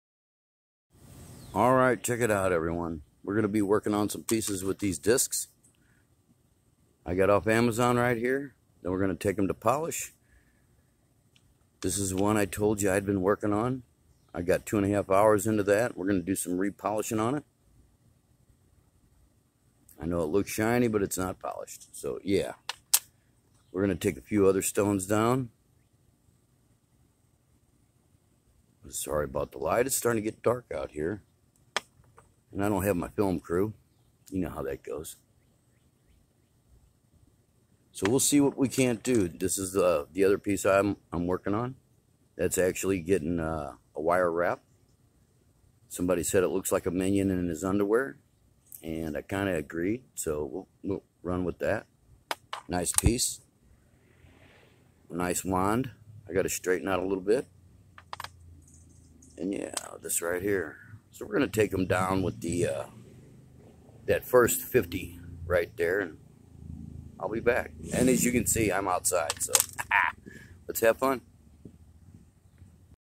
PORTABLE POLISHING PAD SETUP
You will make a mess so be prepared if you're working inside so be prepared.  I'm working in the back yard on my glass table and it works great.  you do have to secure it to the table or have something you can push the board up against.  Back to working on these pieces.